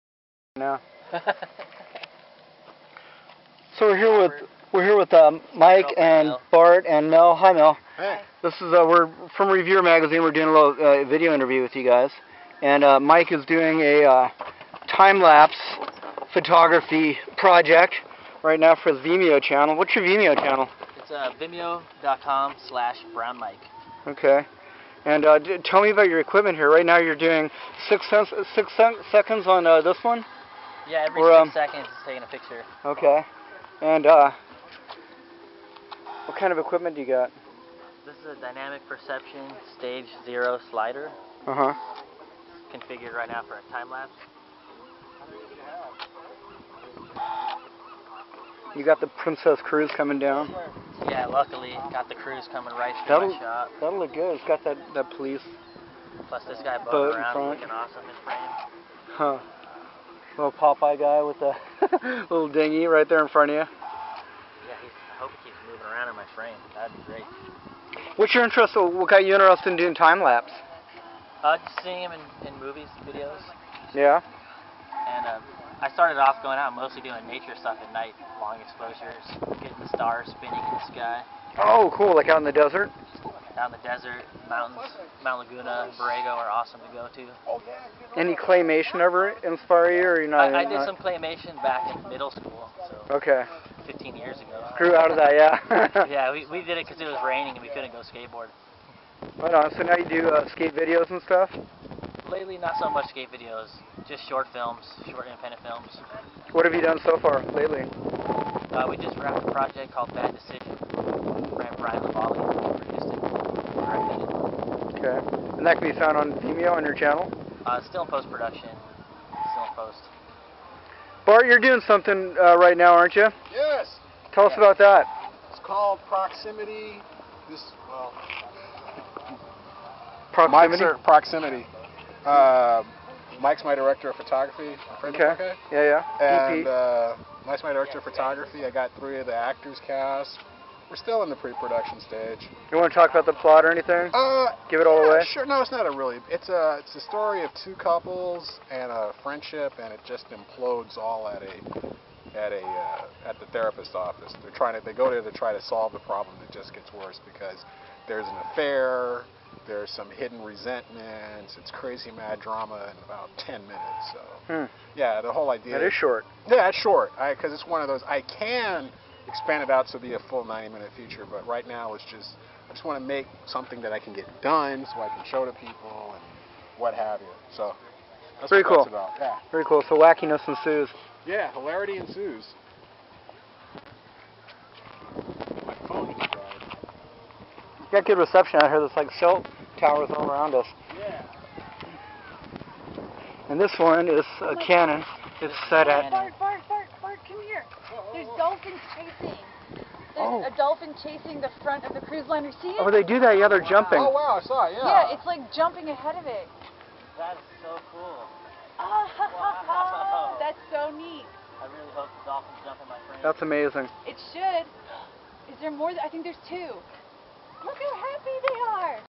Filmmaker Mike Brown Shoots San Diego Harbor
[Note: this is the full re-upload of a previous video clip that viewers here were given a peek of.] Filmmaker Mike Brown shooting time-lapse video and still photography at San Diego Harbor on February 9, 2013. Photographer Bart Cameron and Mike's girlfriend Mel were there too. Mike captured various interactive aspects of the bay including dolphins jumping in the bow wake of the cruise liner leaving San Diego harbor. Bart talks about filming his movie, too, about psychodrama. Very creative people.